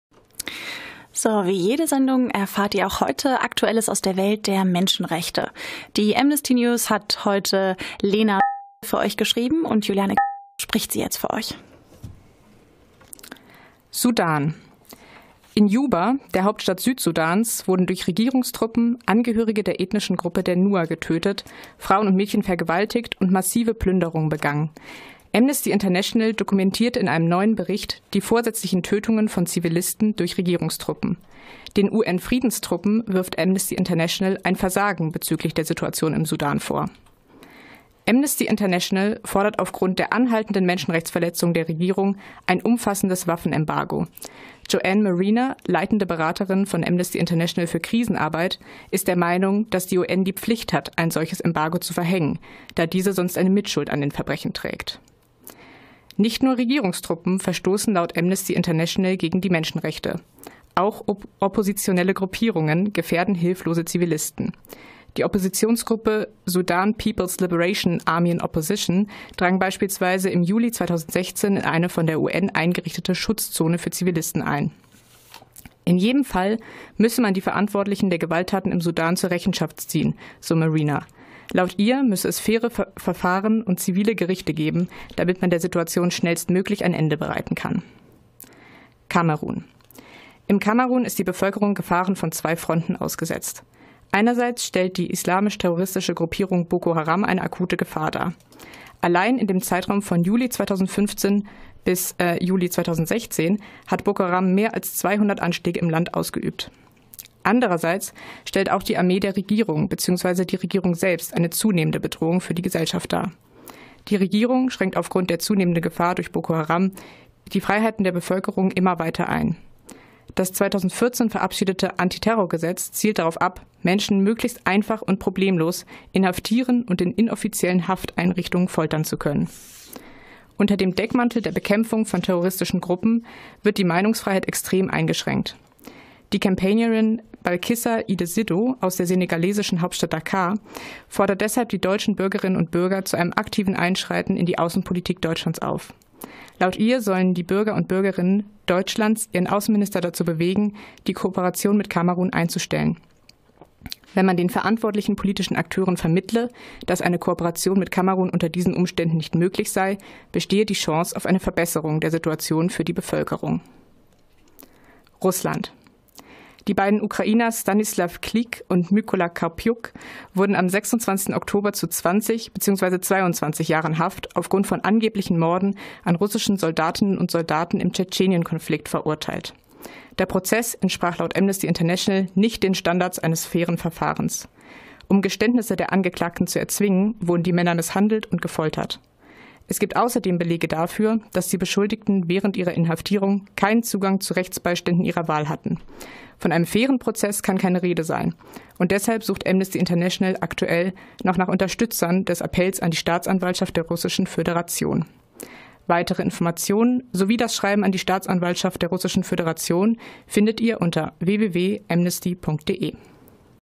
Amnesty News - 7. November 2016
Juliane liest aktuelle Nachrichten aus dem Südsudan, aus Kamerun und Russland.